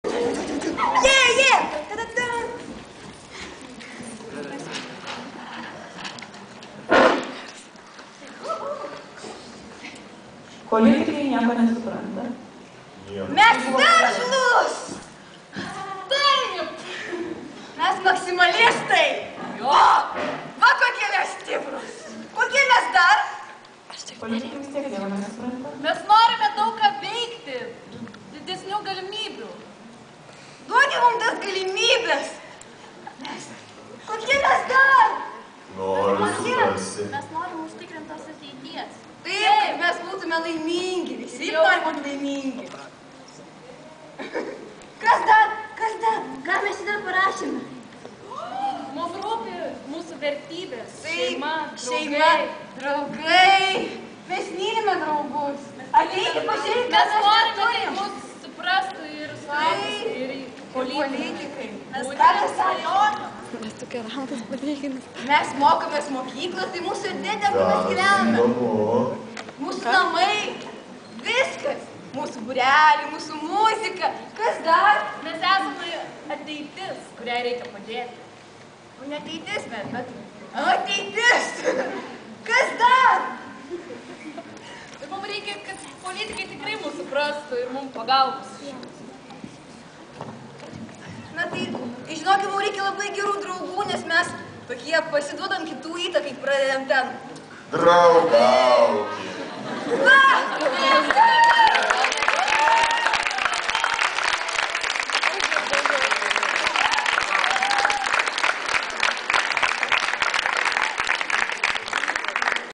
Trasalis
Jame vyko  programa supazindinti jaunima su Jaunimo mainais.